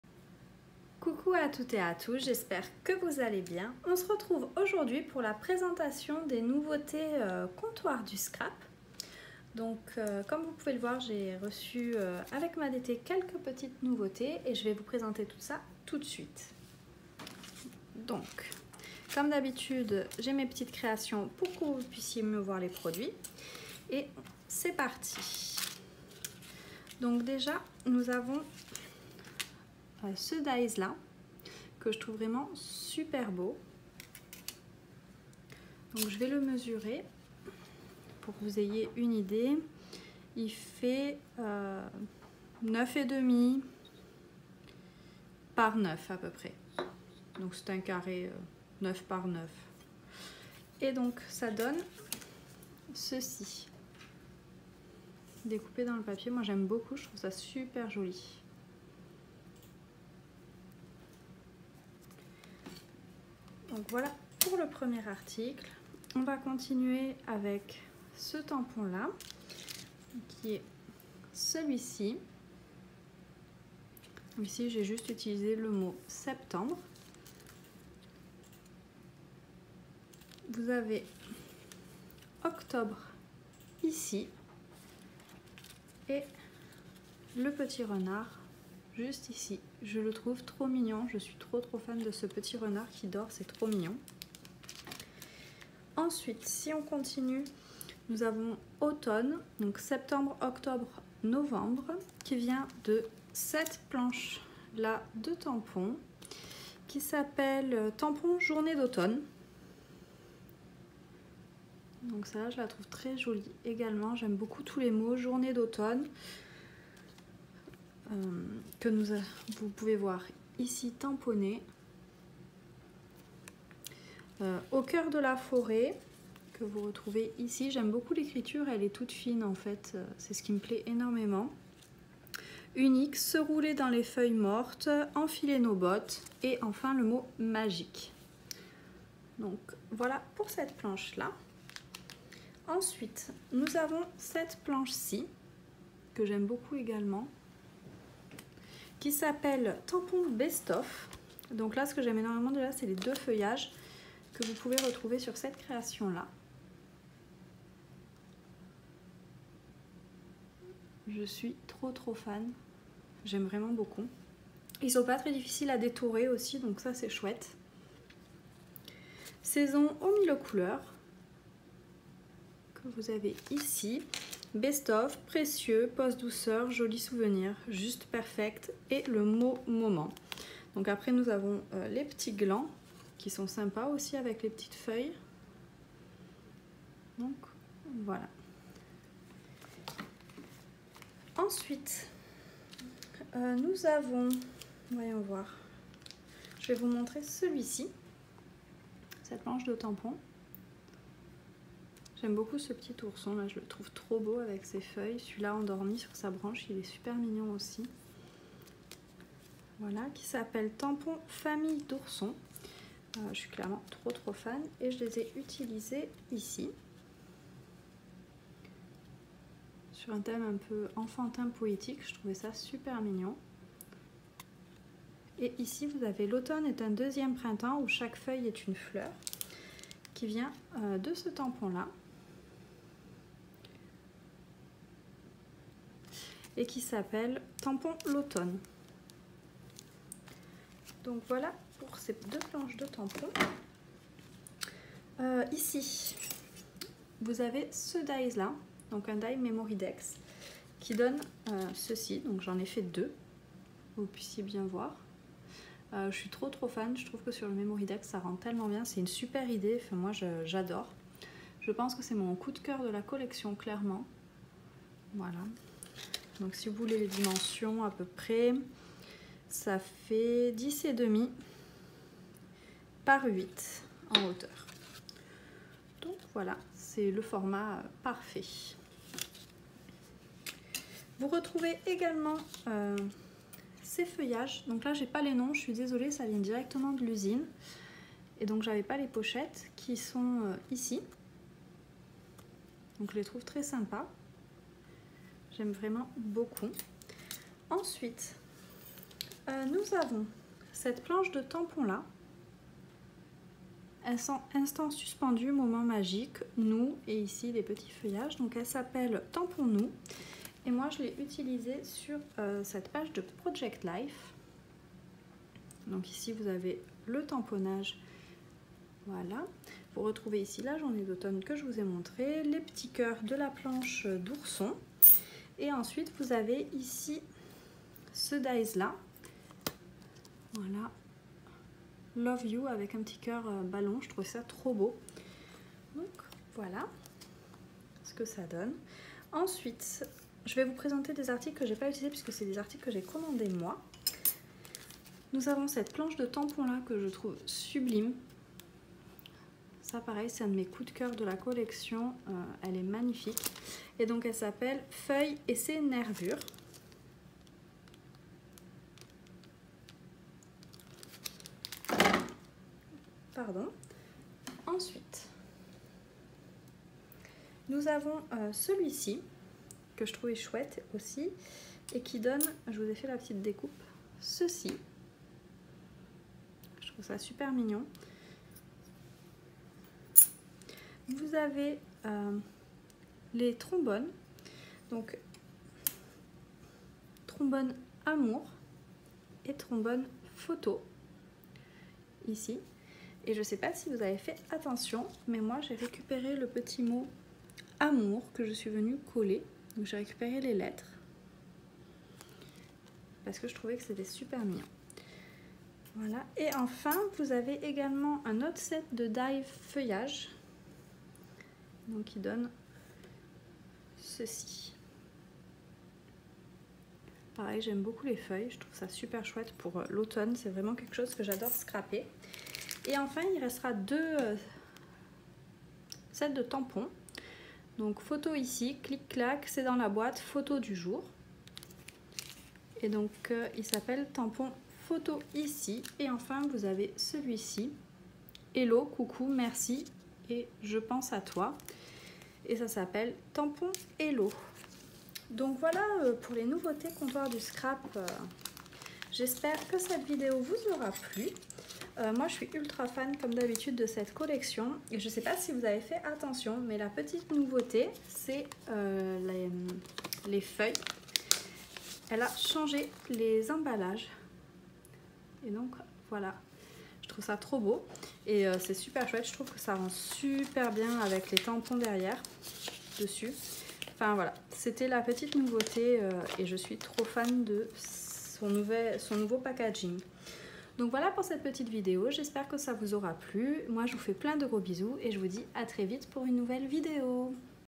Nouveautés Comptoir du scrap septembre 2021
Coucou à toutes et à tous,

Je vous retrouve aujourd'hui pour le unboxing de mon colis DT Comptoir du scrap.
J'espère que cette vidéo t'aura plu ! Si c'est le cas n'hésite pas à laisser un petit commentaire. Merci d'être passé par là !

Plein de bisous et à bientôt pour une prochaine vidéo !